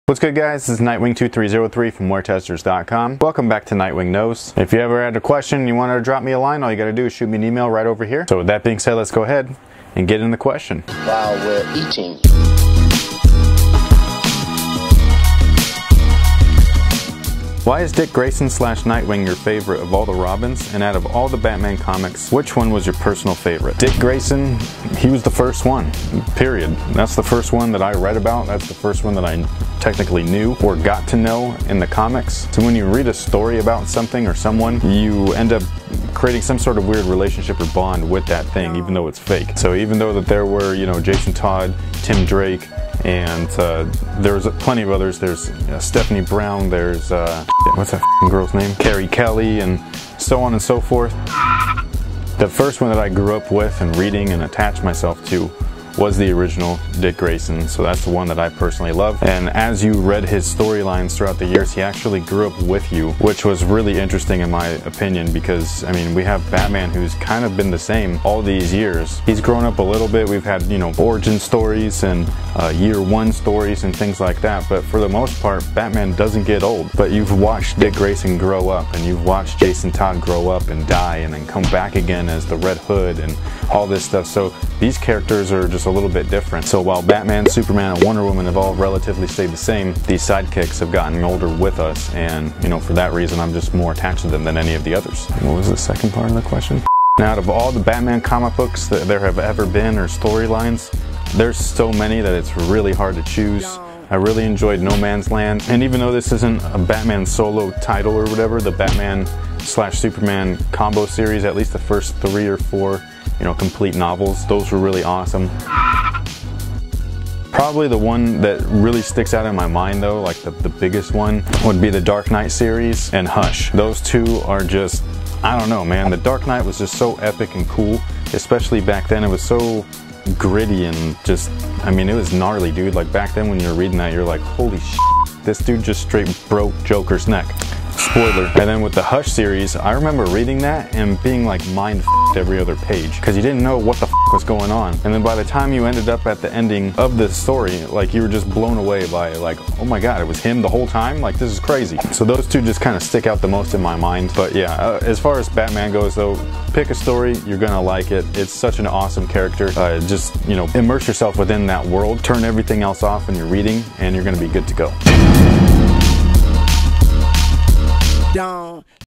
Nightwing Knows | Why is Dick Grayson / Nightwing my favorite Robin ?
Hey Guys! Thank you for tuning in to Nightwing Knows, previously known as askNightwing. I why Dick Grayson/ Nightwing my favorite of the Robin characters, and what were some good Batman storylines to read.